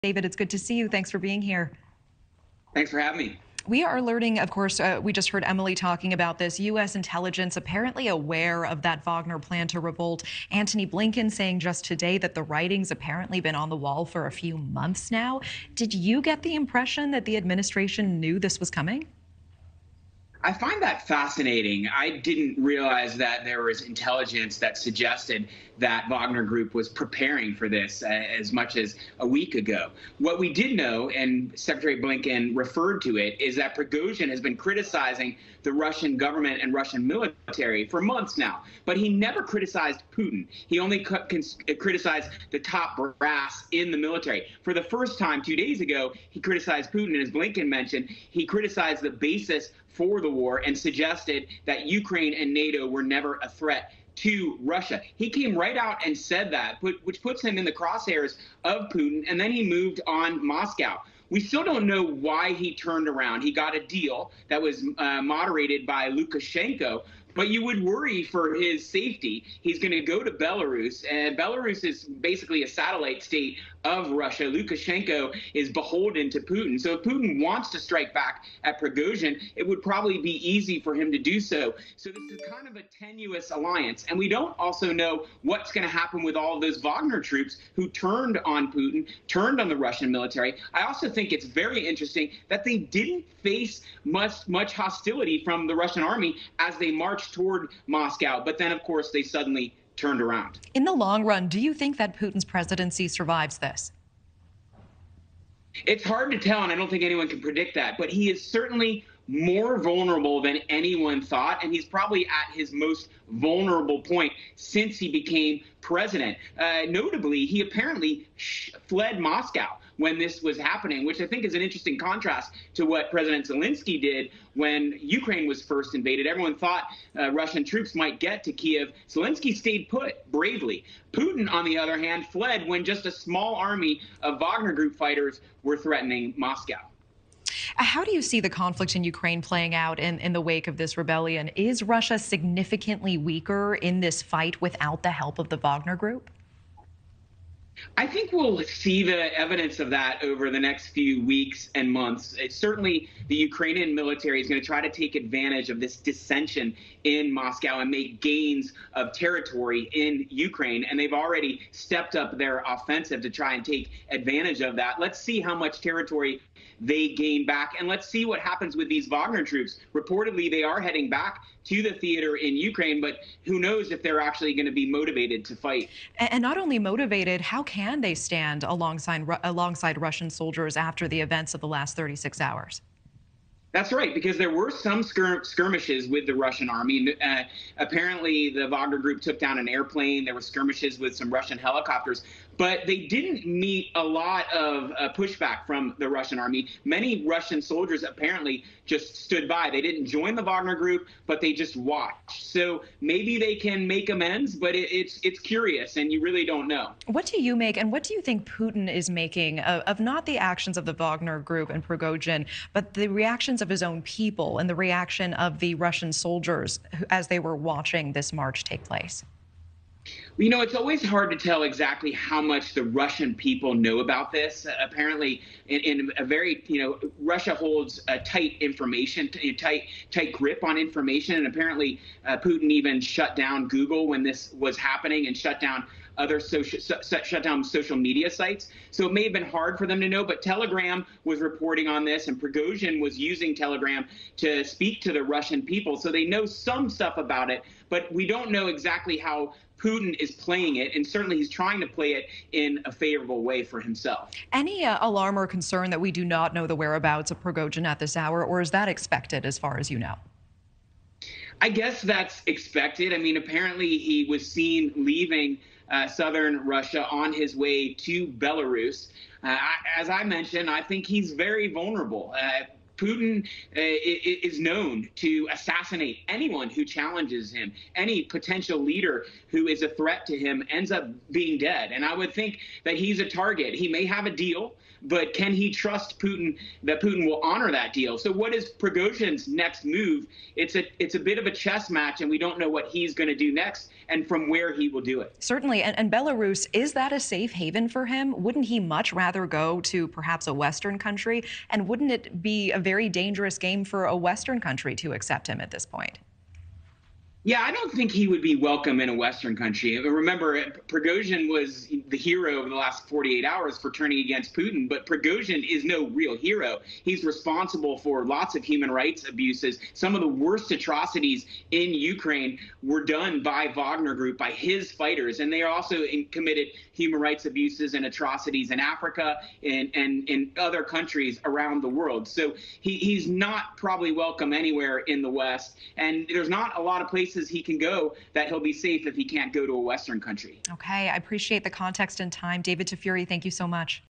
US Intelligence on Wagner Group, whether Putin can survive and what comes next for Prigozhin
David Tafuri with Natasha Zouves on NewsNation on June 25, 2023